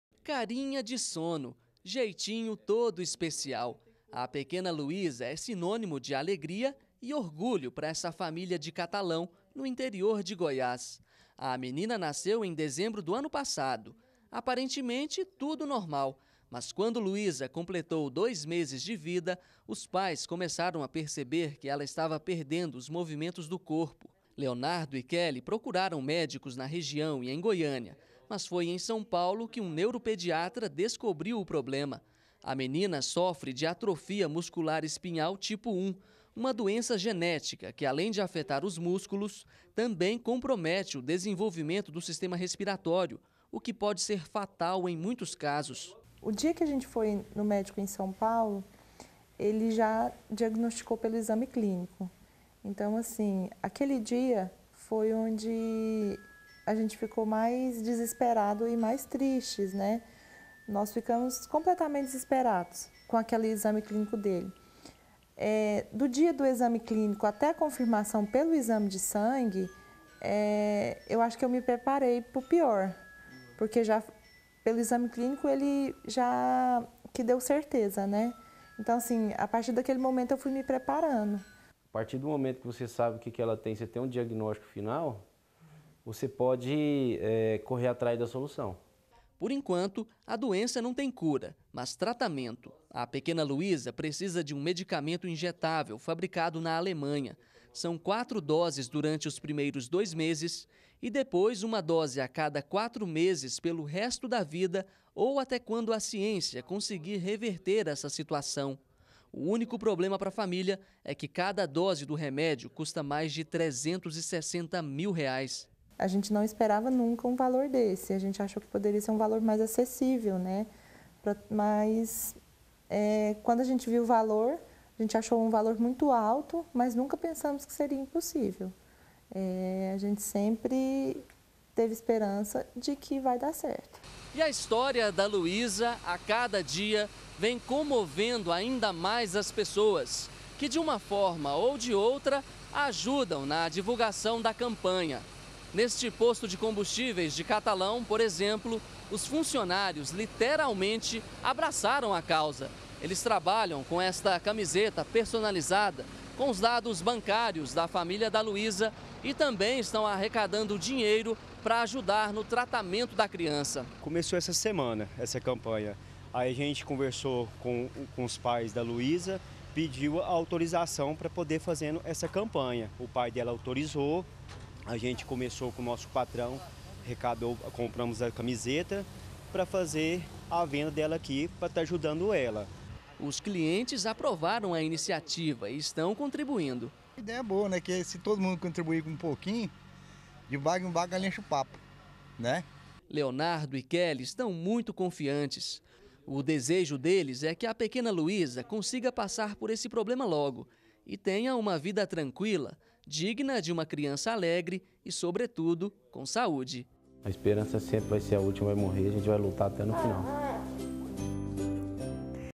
CA - Voluntários mobilizados para ajudar Luisa - 19-04-2018
Muitas pessoas estão mobilizadas para ajudar no tratamento da pequena Luisa, de Catalão. A bebê tem apenas quatro meses e sofre de atrofia muscular espinhal. O tratamento é muito caro, cada dose do remédio custa mais de 350 mil reais.